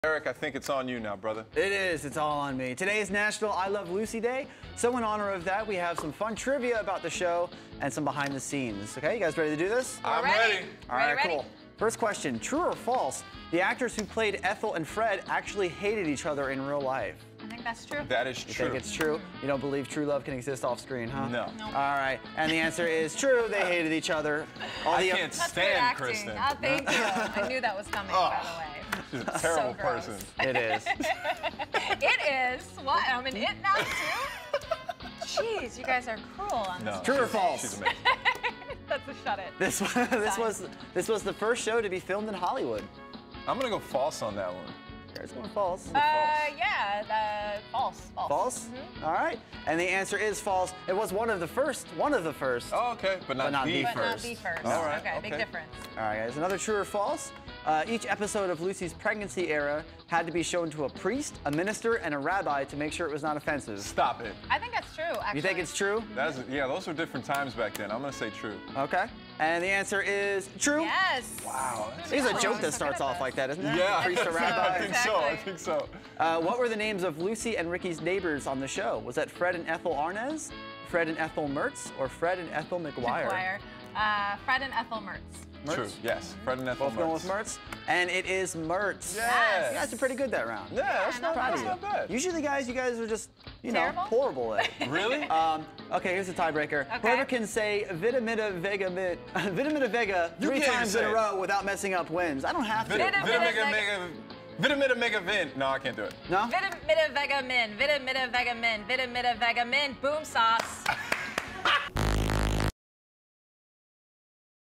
Wacky Wednesday Trivia: Lucille Ball Edition!
We love Hump Day here at the Daily Buzz, so we celebrate the way we know best: with Wacky Wednesday Trivia! And since it's National Lucy Day, we're celebrating with trivia about the iconic 50s television show!